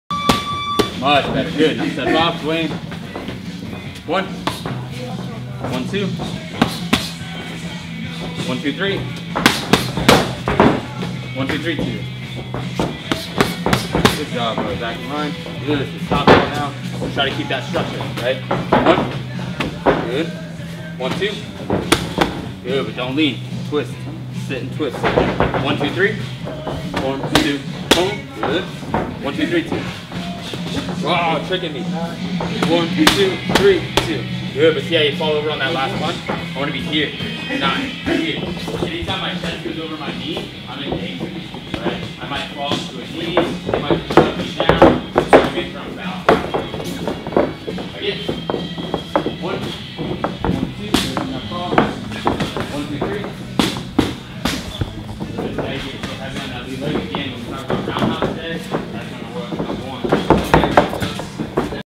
Coach Rich Grindle Of Glory Martial Arts
"A quick vlog of Glory Martial arts 7 P.M  Striking class with coach Rich Grindle.'